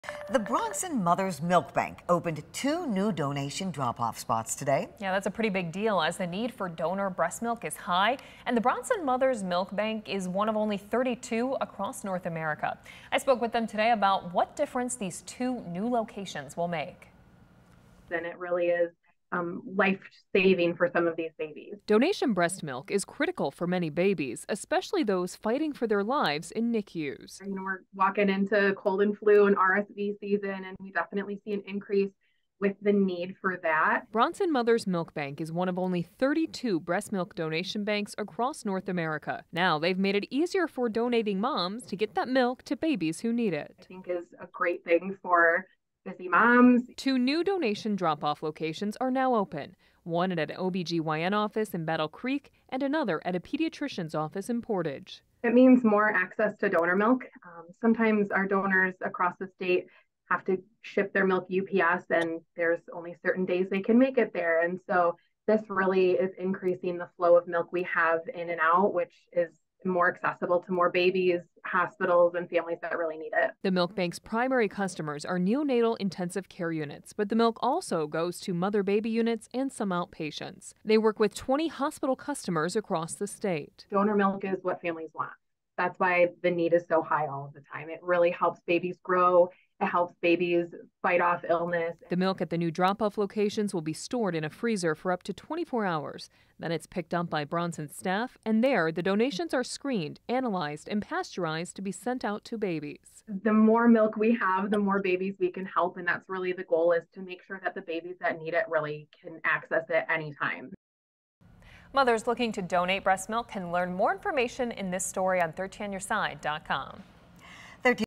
West Michigan breast milk bank opens two new drop-off locations for donating moms
New locations in Portage and Battle Creek make it easier for moms to donate breast milk.